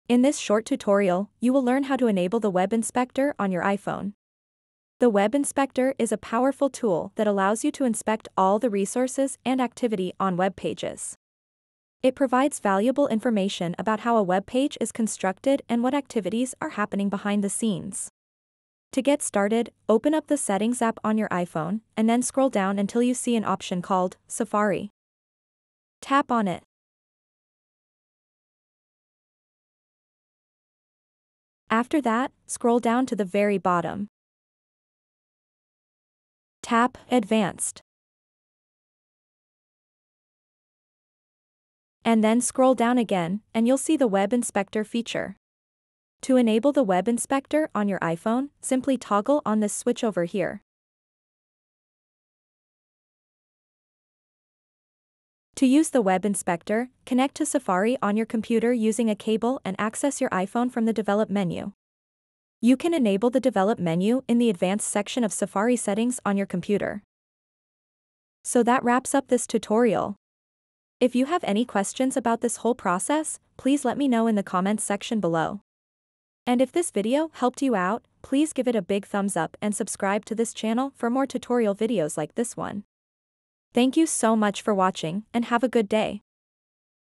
How To Enable Web Inspector On iPhone
In this short tutorial, we will show you how to enable the web inspector on your iPhone. The web inspector is a powerful tool that allows you to inspect and debug web content on your device. By following a few simple steps, you can easily enable the web inspector on your iPhone and take advantage of its powerful debugging and analyzing capabilities for web content.